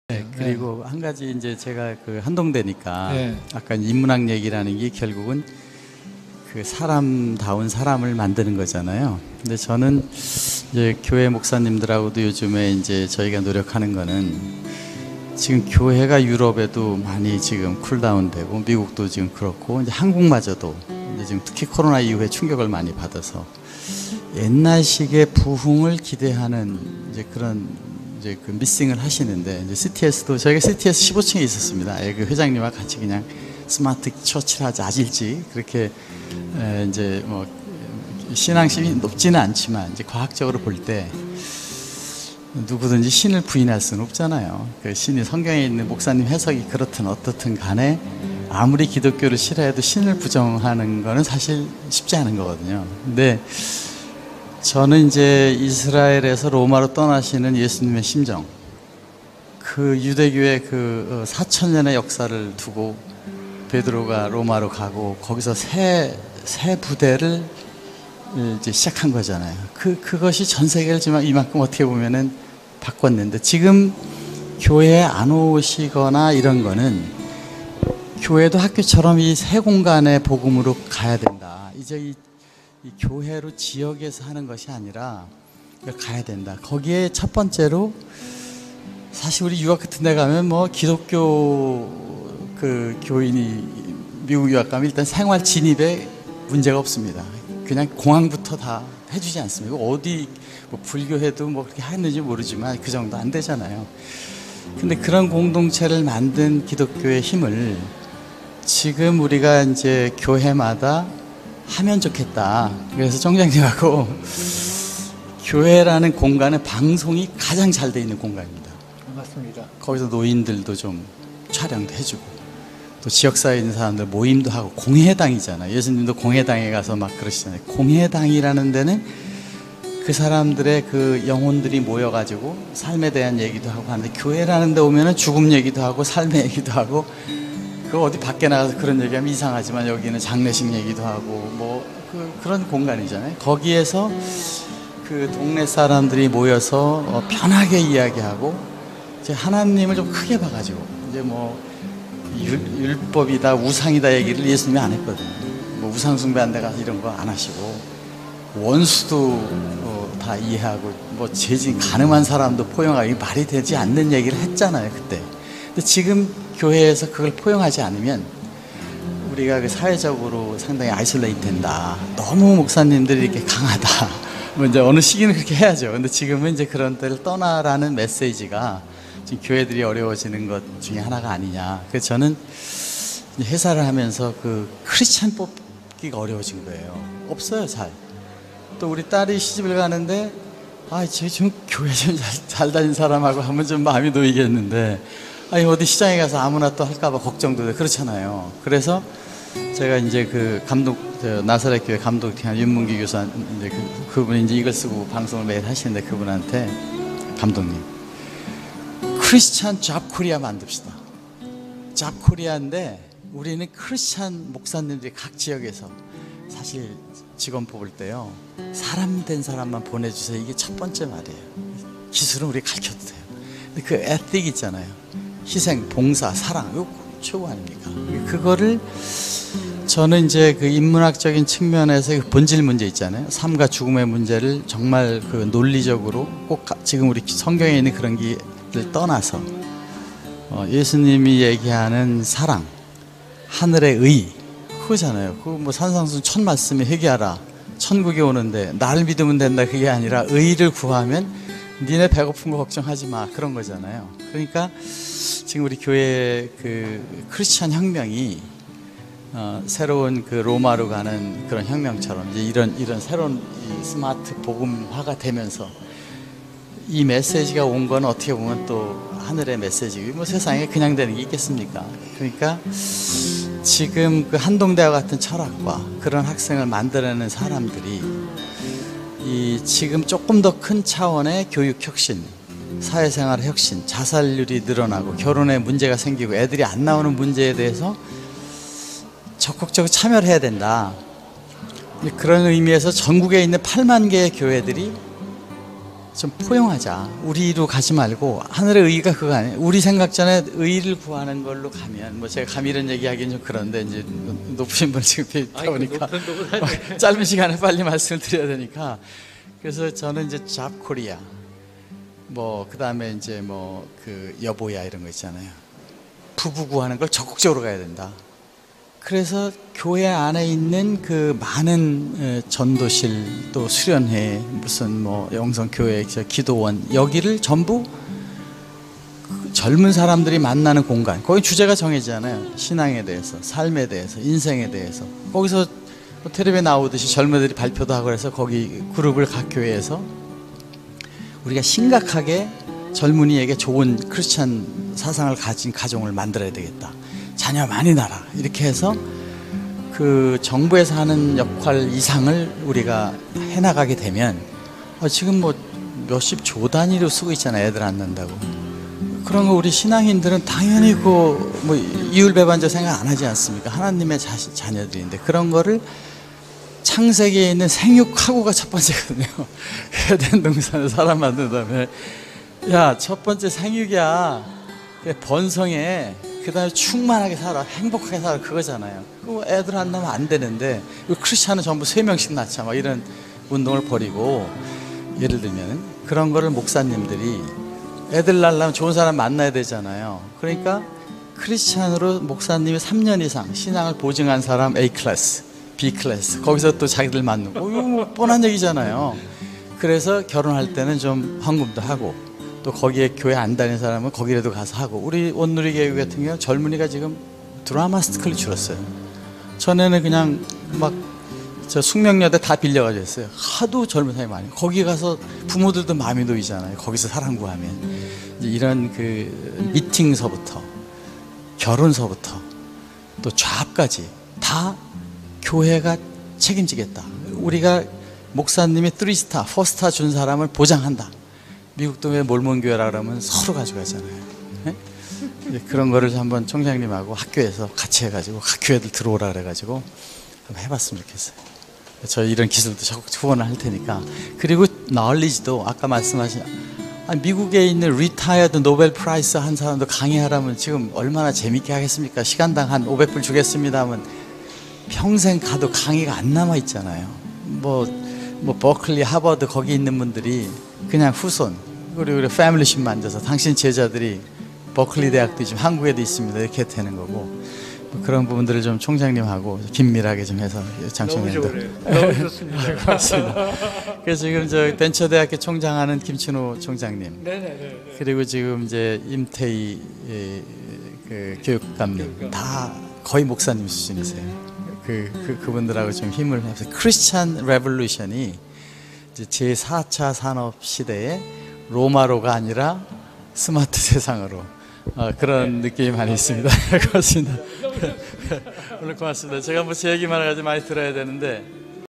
한동대 최도성 총장님과 밀레니엄 시대의 메타버스 교회의 엑소더스 이야기
에굽에서 가나안으로, 예루살렘에서 로마로,, 새 밀레니엄의 기독교의 MetaVerse로의 엑소더스는 율법에서 기독 복음으로의 혁신과 같은 새로운 기독인의 사랑의 스마트 세상의 사랑의 새 에덴의 기독 세상의 구현을 위한 예수님의 이방인도 사랑하는 포용의 기독교 사랑으로 원수도 사랑하라는 말씀처럼 세상의 모든 어두운곳까지 새롭게 하는 미션을 기대해 본다.